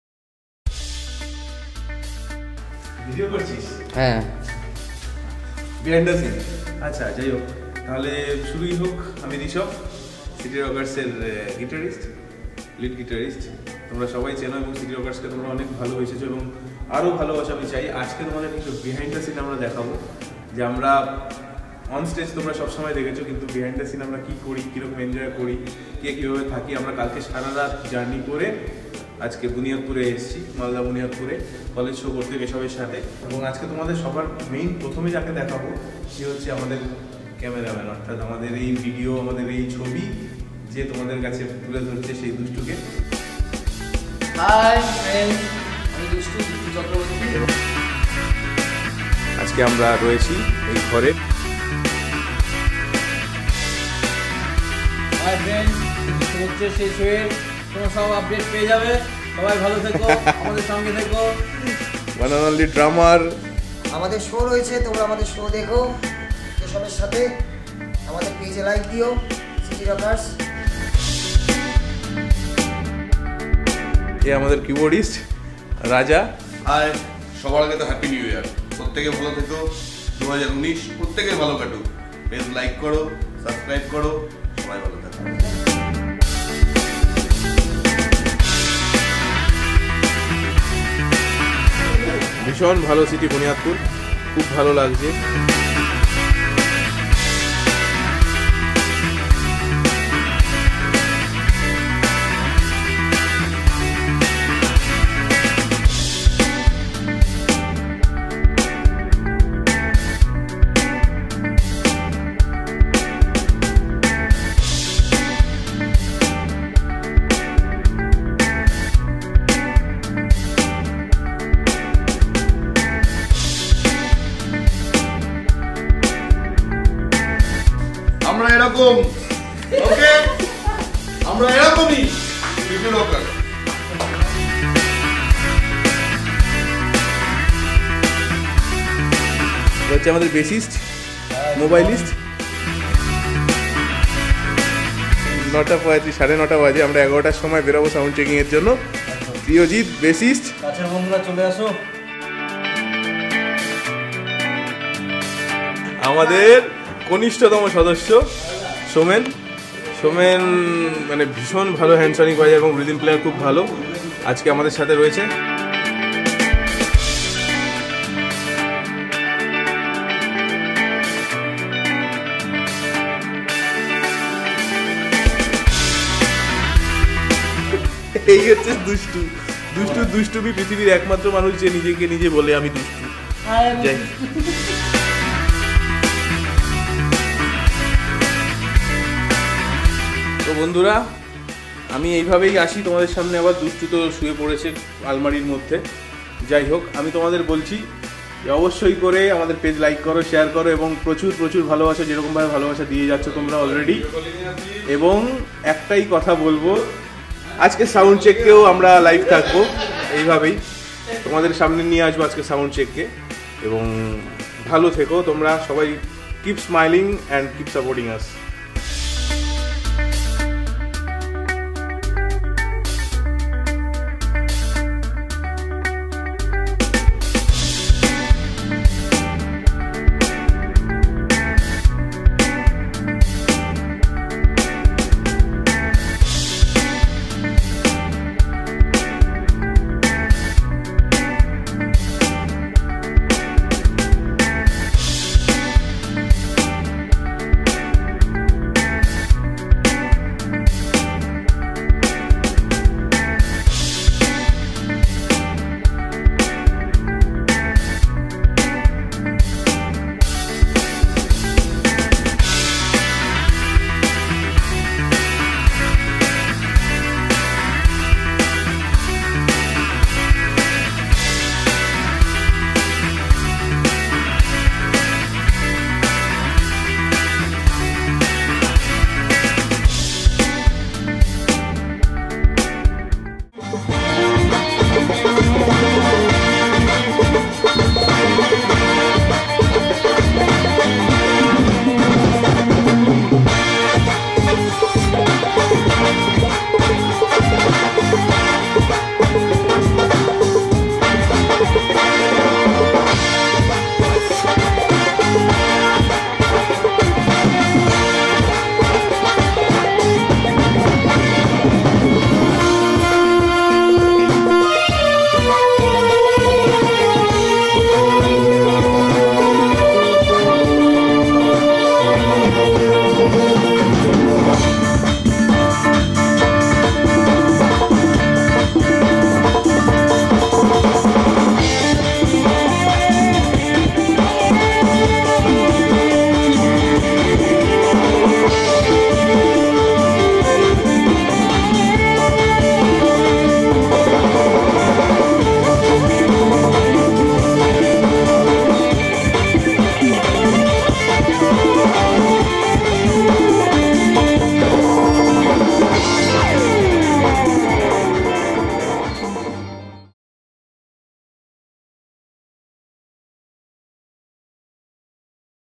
City Rockers Behind The Scenes Masti | Sound Check | Malda Buniadpur College
City Rockers Presents Behind the Scene of Malda Buniadpur Mahavidyalaya (College)

For Show Booking : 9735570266 Rishav / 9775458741 Pol / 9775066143 Raju